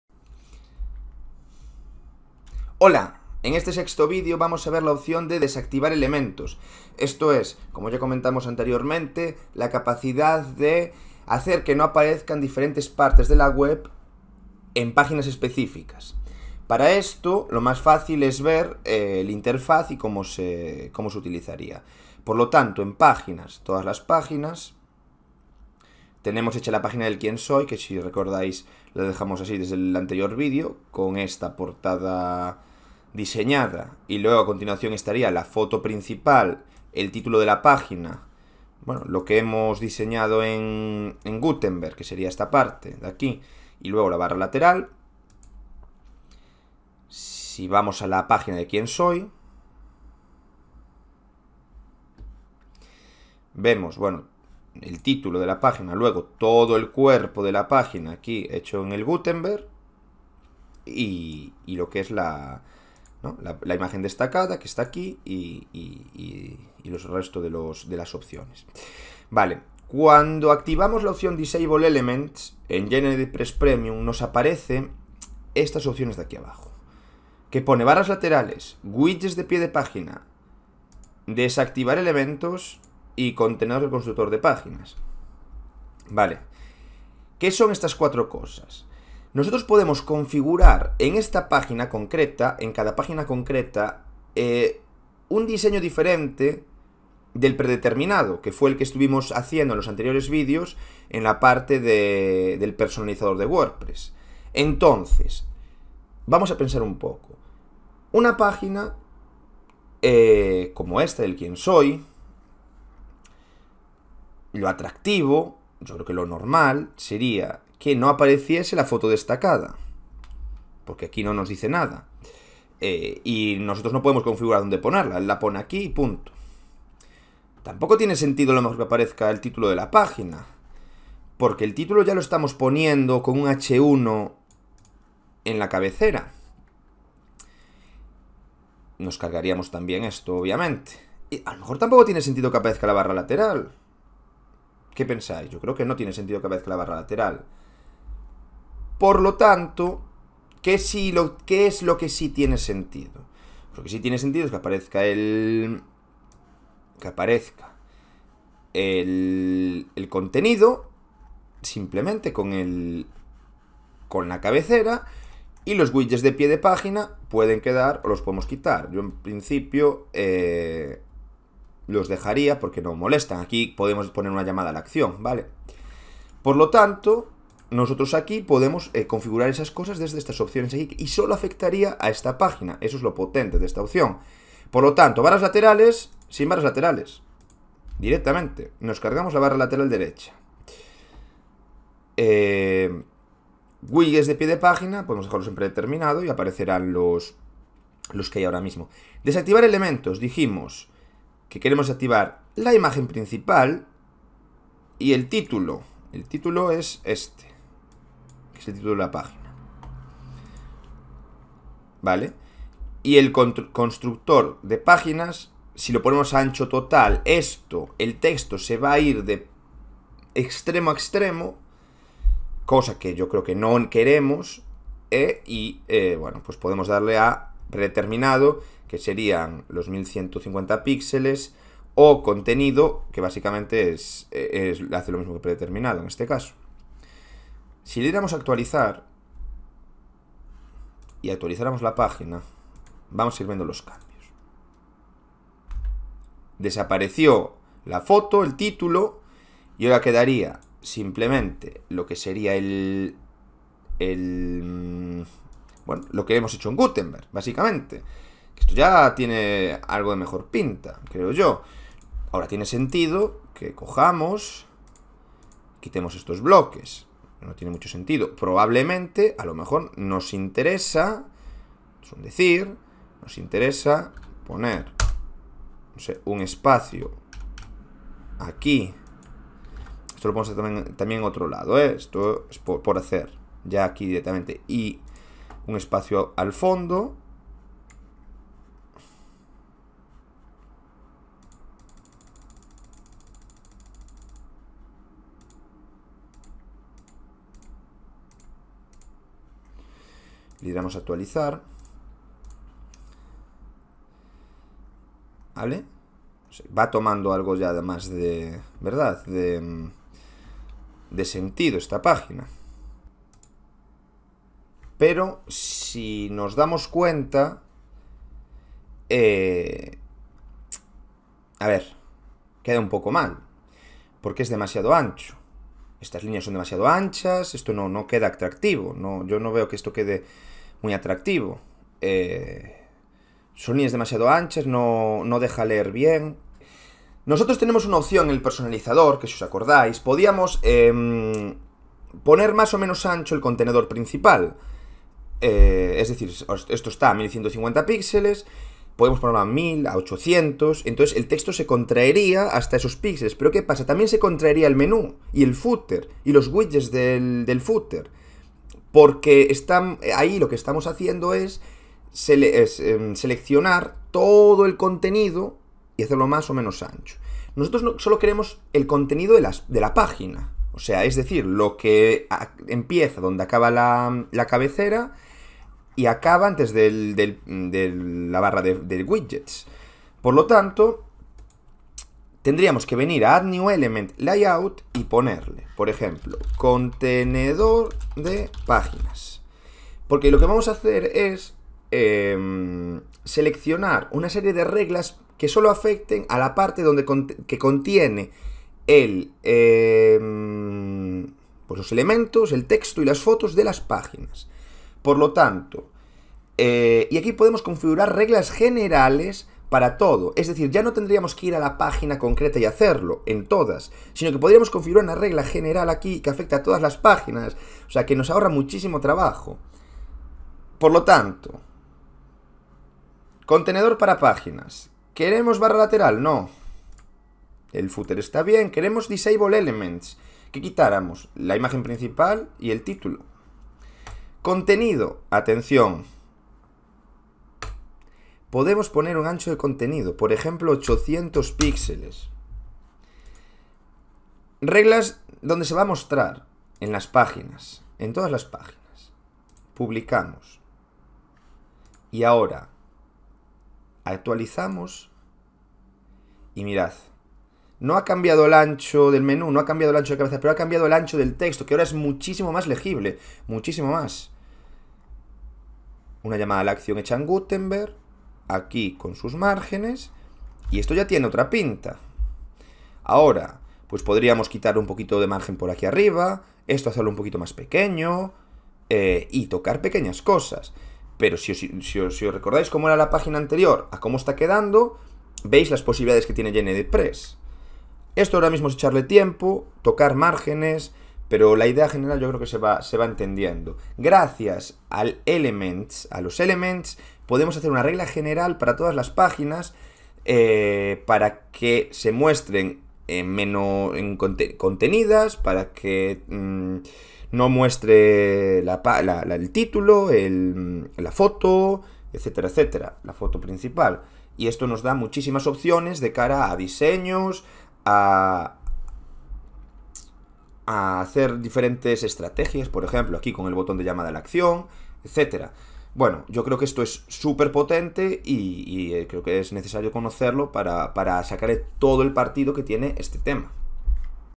🚫 GeneratePress ▶ DESACTIVAR ELEMENTOS del MEJOR tema de WordPress · #006
Puede que en algunas páginas queramos desactivar algunos elementos, como las barras laterales, el título o la imagen destacada, por razones de diseño o estrategia. Gracias a esta opción se puede hacer, manteniendo el estilo del resto de las páginas intacto.